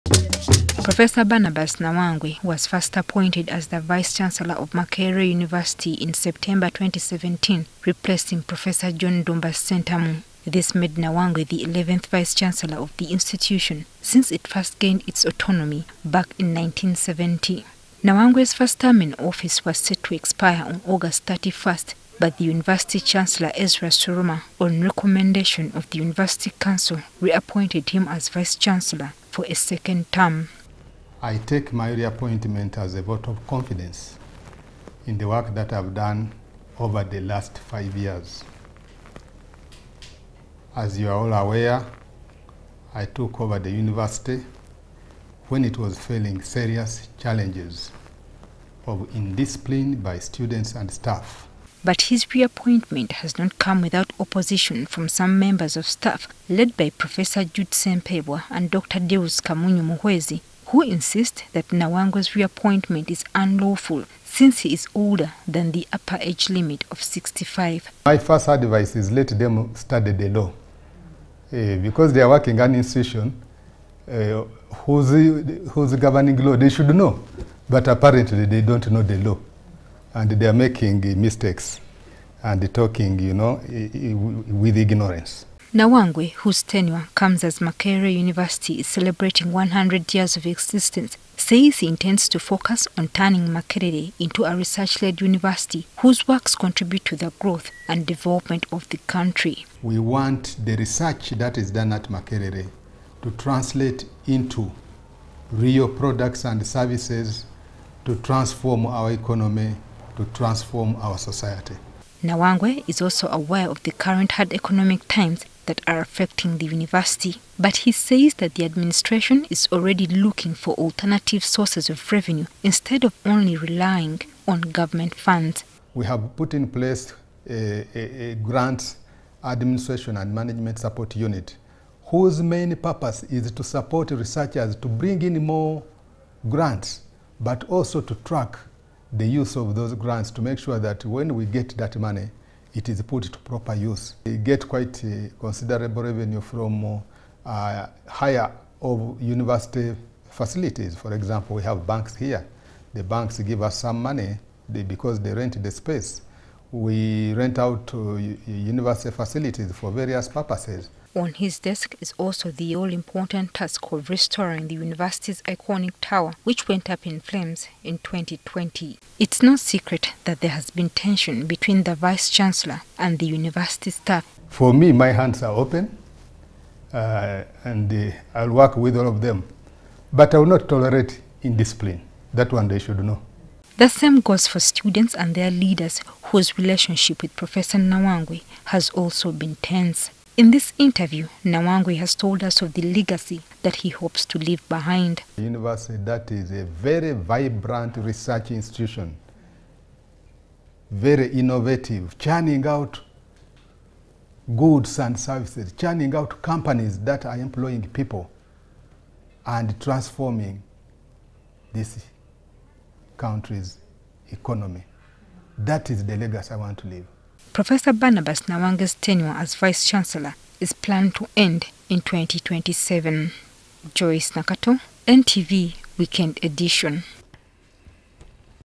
Vice Chancellor Barnabas Nawangwe says he is committed to the job
Following his reappointment as Vice Chancellor of Makerere University, Prof. Barnabas Nawangwe has pledged to use his next five years in office to turn the institution into one of the world’s biggest research institutions. According to Prof Nawangwe,  high quality university research when turned into a real product can transform economies and societies at large, something needed as the world emerges from the grip of Covid-19. On Friday, the university council, led by Lorna Magara, announced Prof. Nawangwe’s reappointment for a second term as vice chancellor.